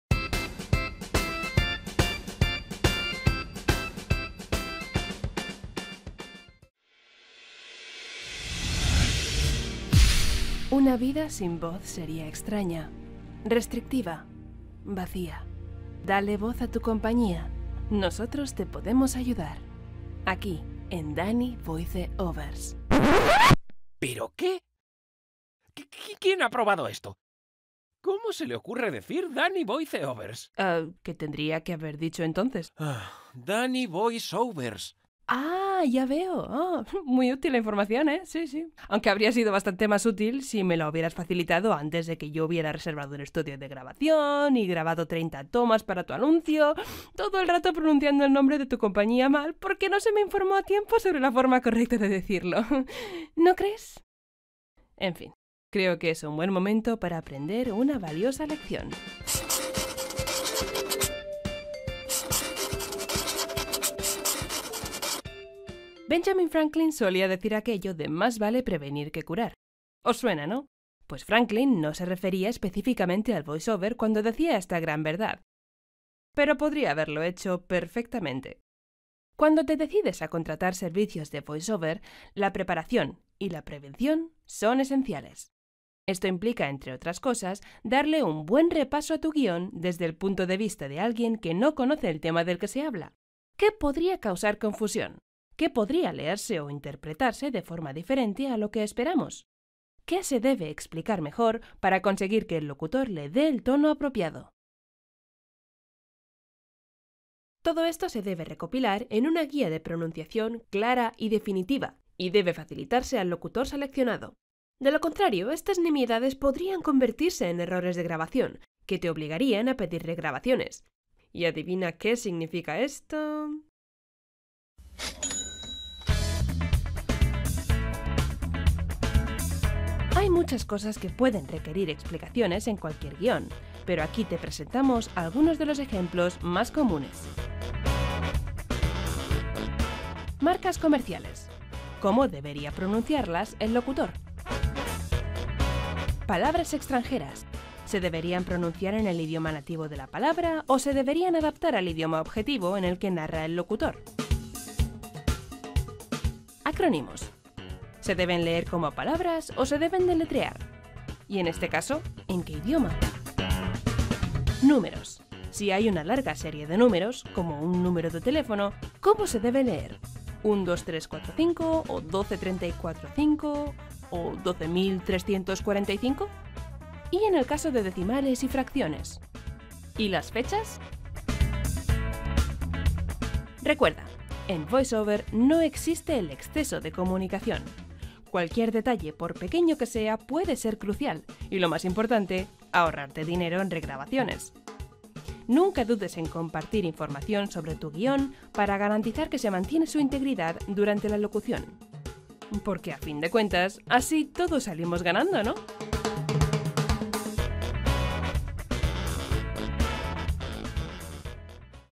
Locuciones profesionales: la Guía de Pronunciación
Cuando te decides a contratar servicios de Voice Over, la preparación es esencial. Esto implica darle un buen repaso a tu guión, ver qué podría causar confusión o leerse de forma diferente. Todo esto se debe recopilar en una Guía de Pronunciación clara y definitiva.